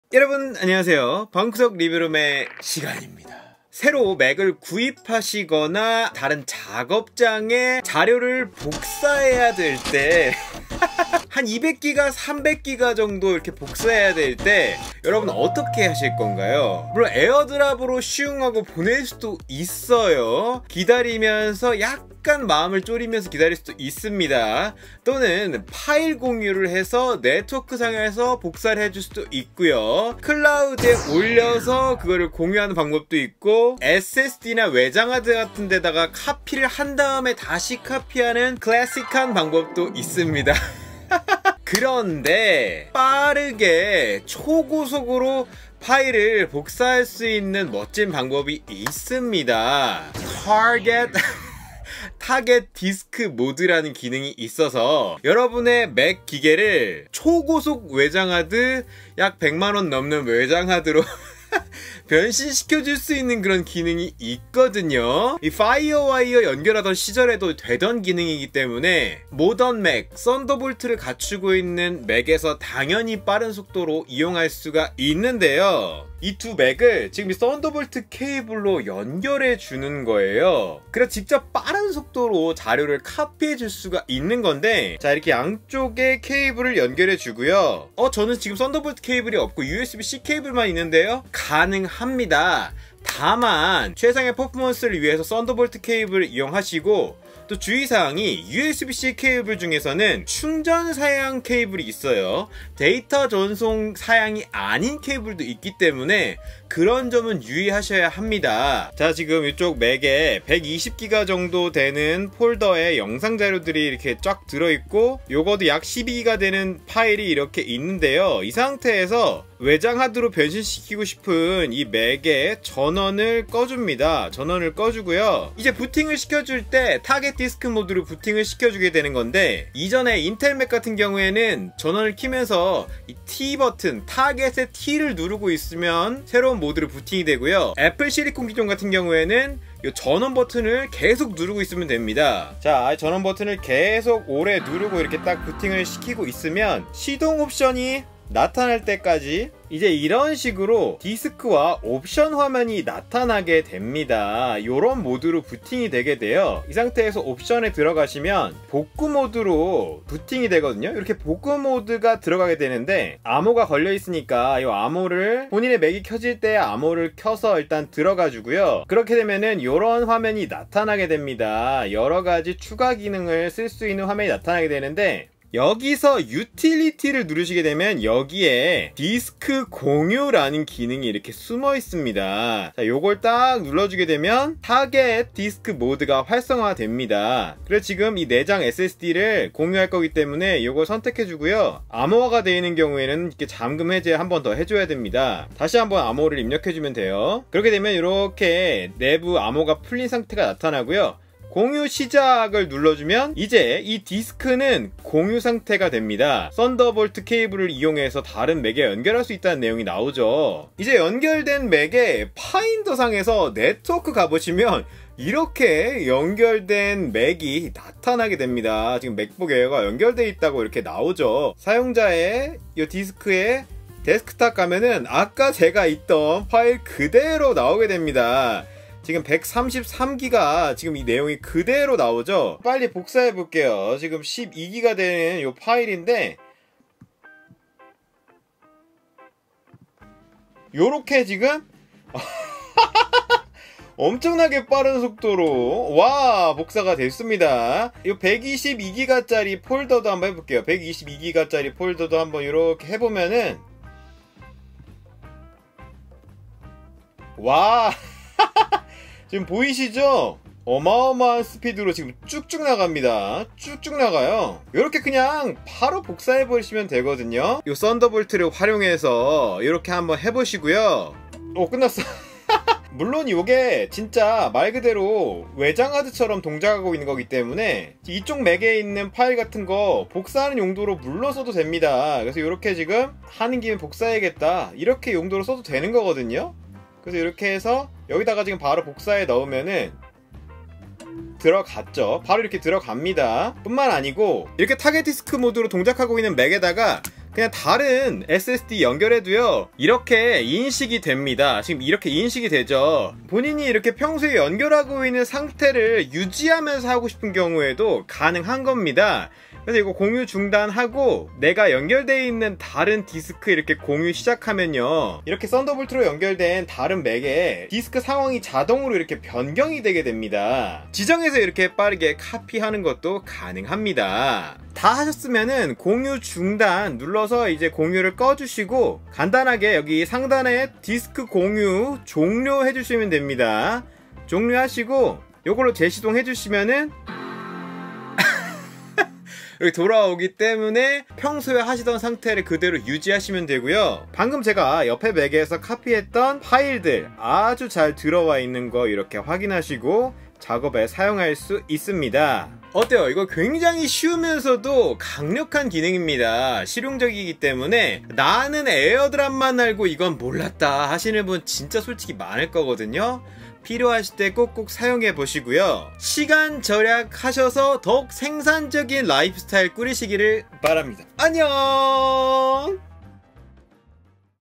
에어드롭 말고 이거! 몇백기가 파일도 초고속으로 전송하는 방법
몇 백기가에 달하는 대용량 자료를 다른 mac에 복사해야 할 때 어떤 방법을 써야할지 모르신 경험 있으실겁니다. 에어드롭을 사용하거나 클라우드에 업로드해서 다운로드 공유를 활용하시는 경우도 있고 대용량의 SSD나 외장하드를 활용하기도 하는데요.
간편하게 다른 맥에 초고속 자료전송이 가능한 타겟 디스크 모드 기능을 활용하면 썬더볼트의 이점을 살린 파일복사가 가능합니다. mac을 외장 드라이브 처럼 인식시키고 대용량의 자료를 복사해서 자료 전송이 가능하니까 알아두시면 큰 도움이 될겁니다.